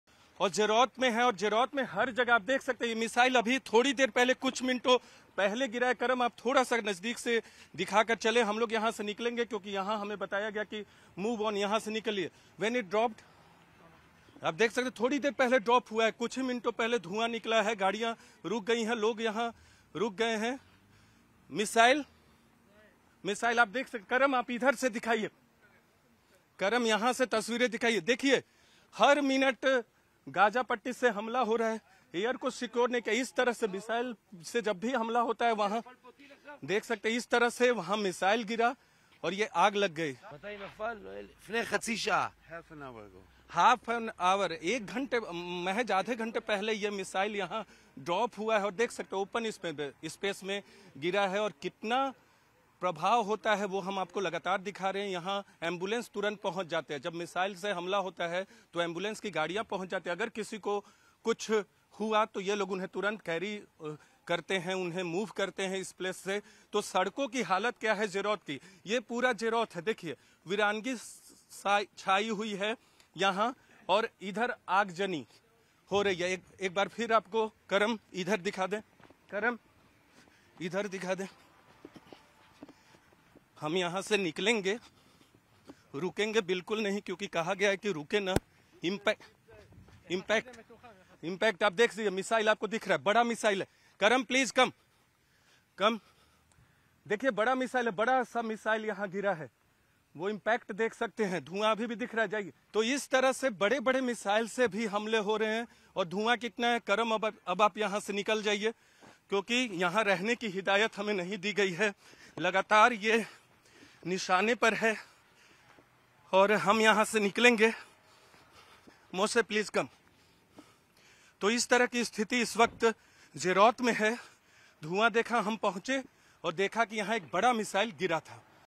Israel Palestine War : बमबारी के बीच फंसा News18 Reporter ! Breaking News । Hamas Israel Attack
Israel Palestine War जारी है. इस जंग की Live Reporting करने गए News 18 की टीम ने कई दृश्य को अपने कैमरे में कैद किए हैं. देखिए ये खास खबर.